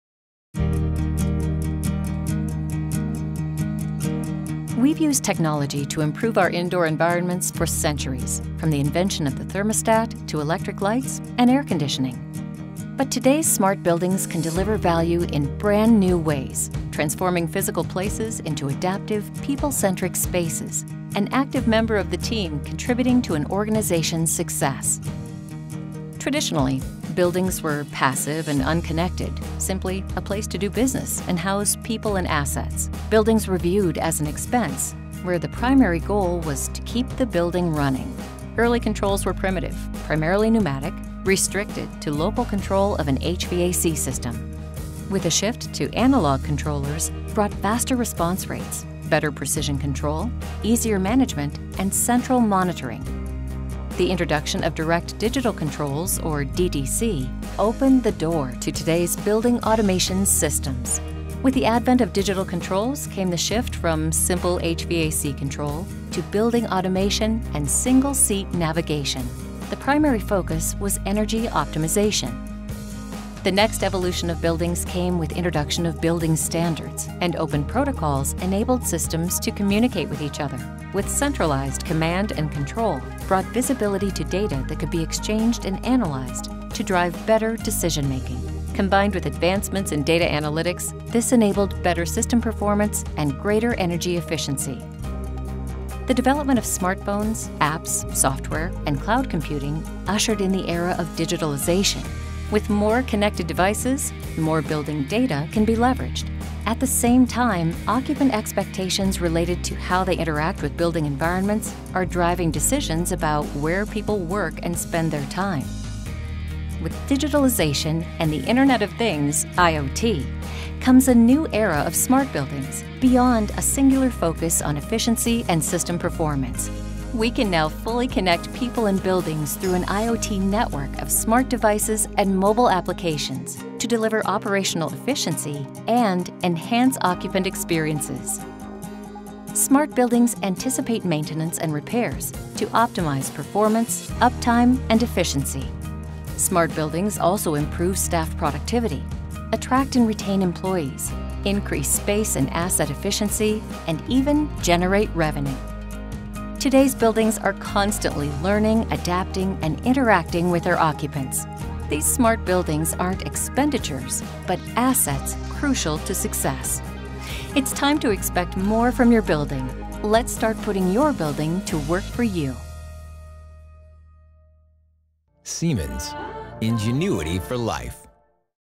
Evolution of Smart Buildings
Traditionally, we’ve thought of buildings as passive and unconnected, a place to do business and house people and assets. These buildings were an expense, inflexible and unable to adapt. They certainly didn’t engage with the people inside. But today’s smart buildings can deliver value in brand new ways: transforming physical places into adaptive, people-centric spaces…an active member of the team contributing to an organization’s success. Siemens understands the evolution of buildings and how to implement technology to make them smart. Learn more about the components of a smart building by clicking